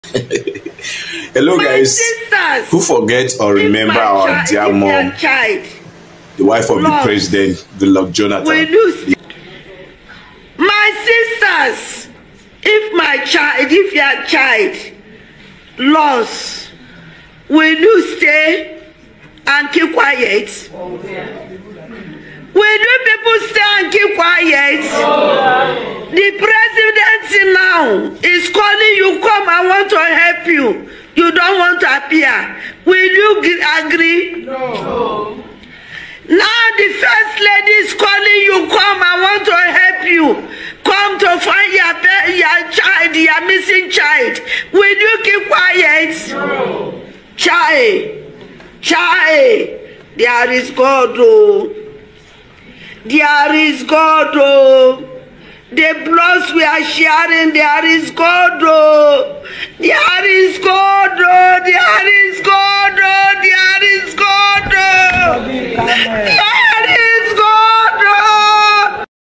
Remembering the Wife of Formal Nigeria President Mrs Patience Jonathan First Lady 😍
This is When Nigeria was Better 😂😂 Mummy First Lady 😍 Who Remember?!